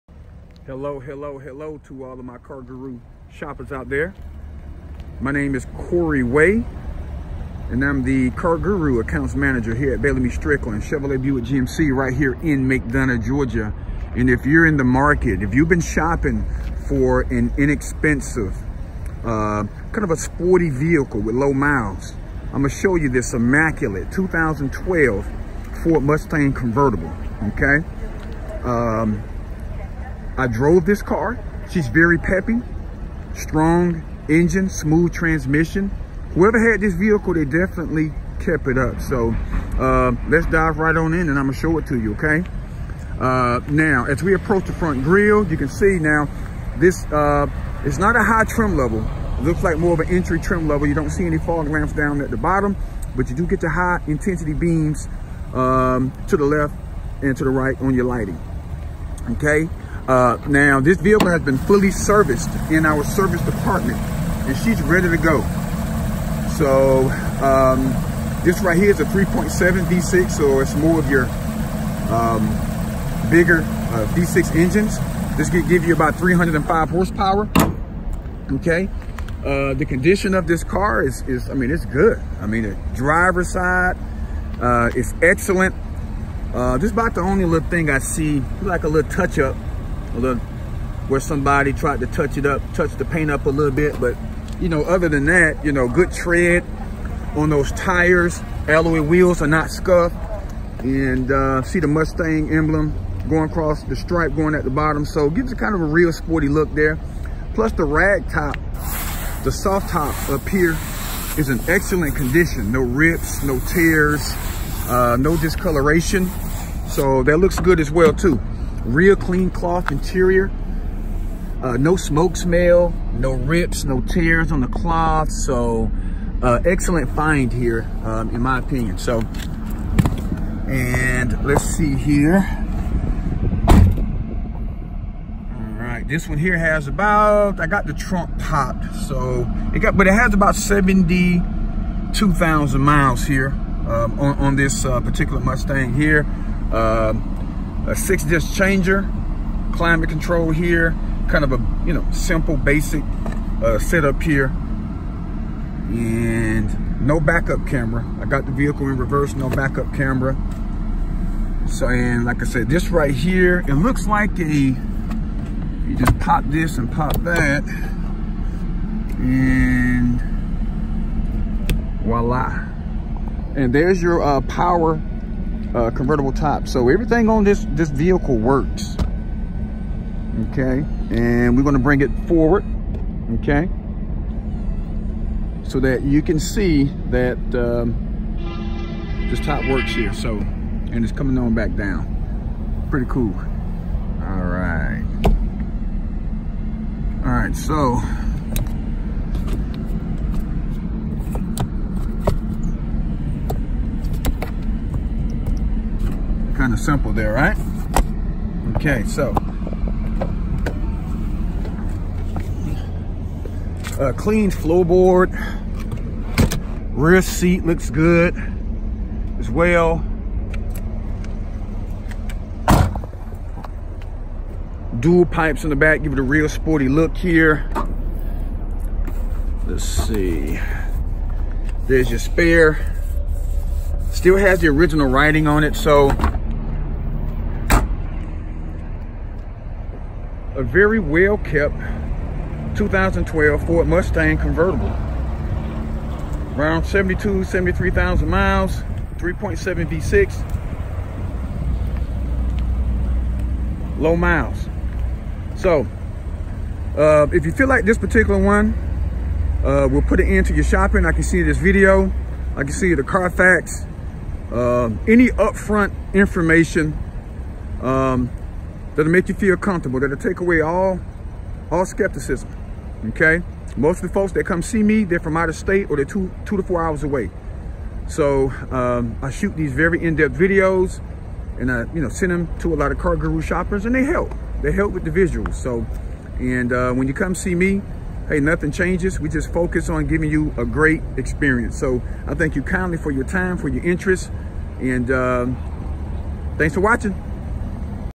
2012 FORD MUSTANG CONVERTIBLE
3.7 V6
White with black cloth
Power convertible top
Cd changer
72k miles